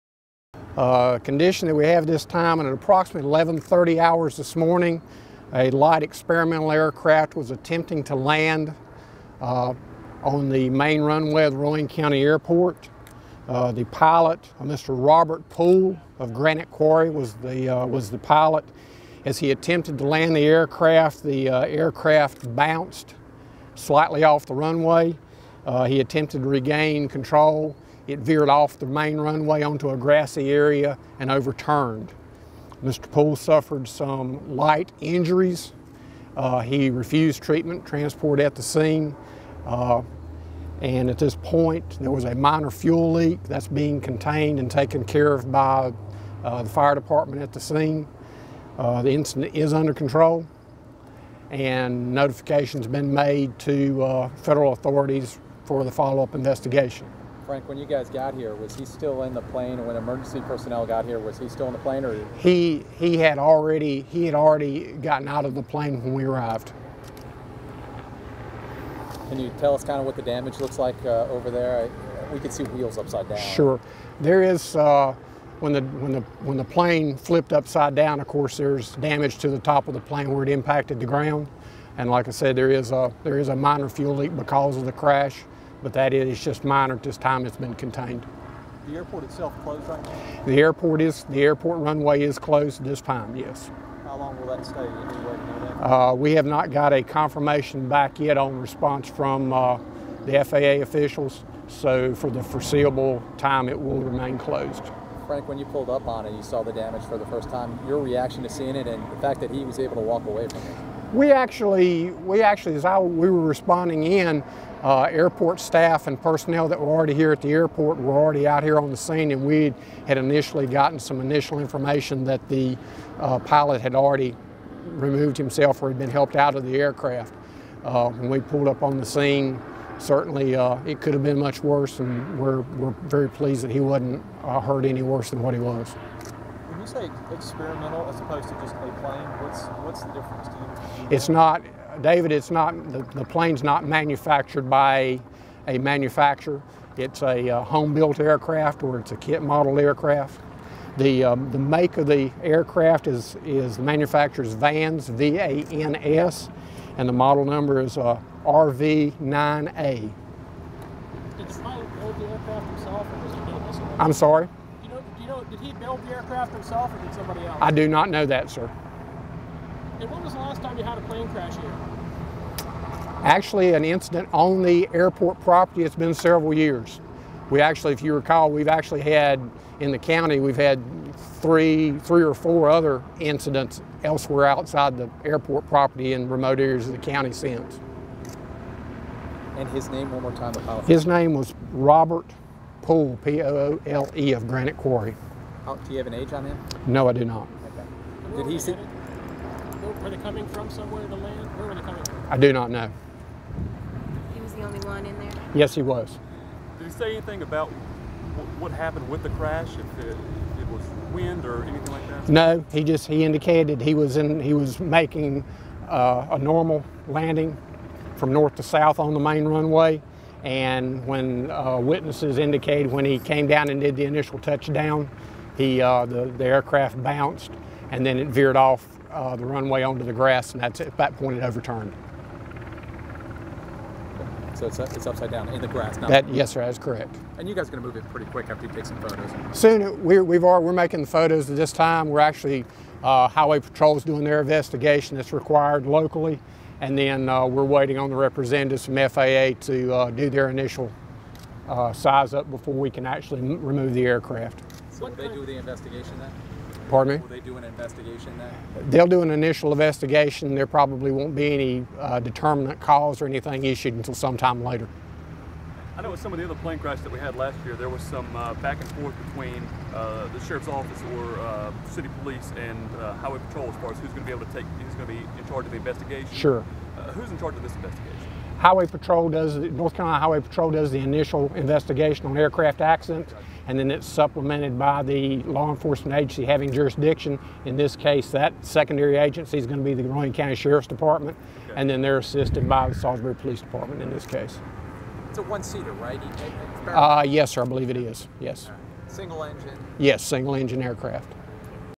Plane crash press conference
Frank Thomason, Rowan County emergency services director, tells reporters about a plane crash at the airport on Wednesday. The plane flipped during a landing, and the pilot walked away with minor injuries.